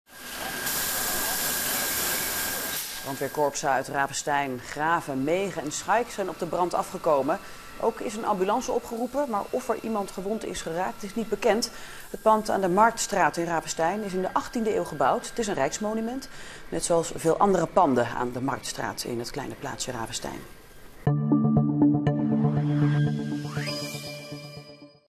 In Ravenstein is donderdagochtend brand uitgebroken in een historisch pand.
Aan de Marktstraat in Ravenstein heeft donderdagochtend een brand gewoed in een historisch pand. De vlammen hebben het hele dak van het Rijksmonument dat uit 1700 stamt verwoest. Waarschijnlijk is asbest vrijgekomen bij de brand. Tien mensen werden geëvacueerd. Niemand raakte gewond.